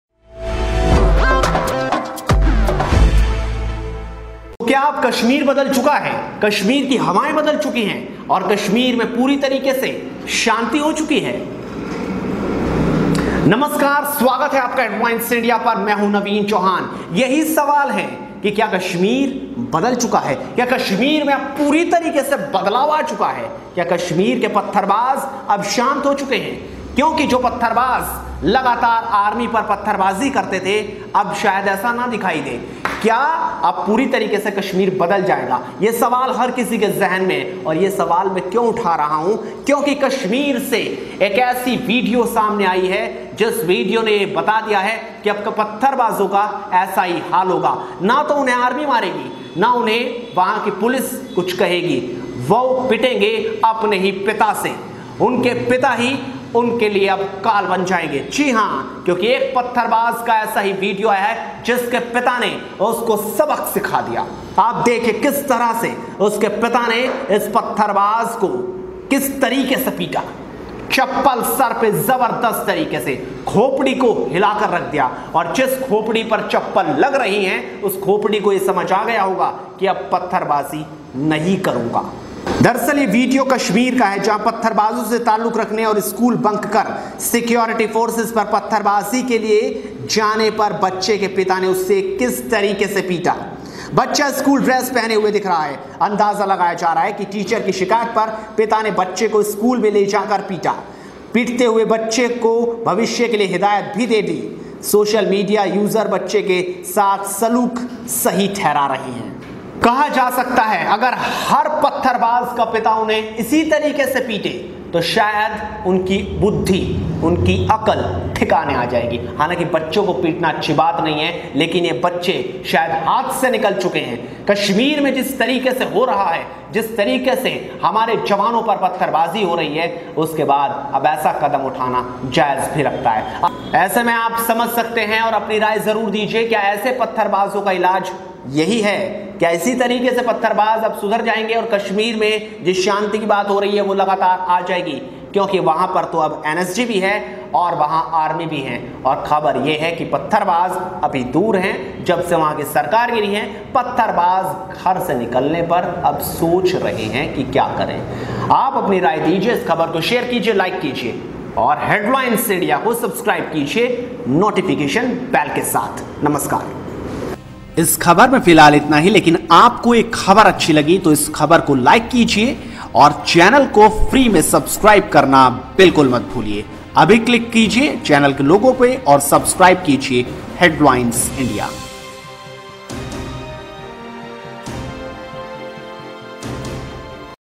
Kashmir में पत्थरबाजों का हो गया एेसा हाल, पिता ने उठाई चप्पल और खोपड़ी कर दी लाल | Headlines India
Kashmir में पत्थरबाजों का हो गया एेसा हाल, पिता ने उठाई चप्पल और खोपड़ी कर दी लाल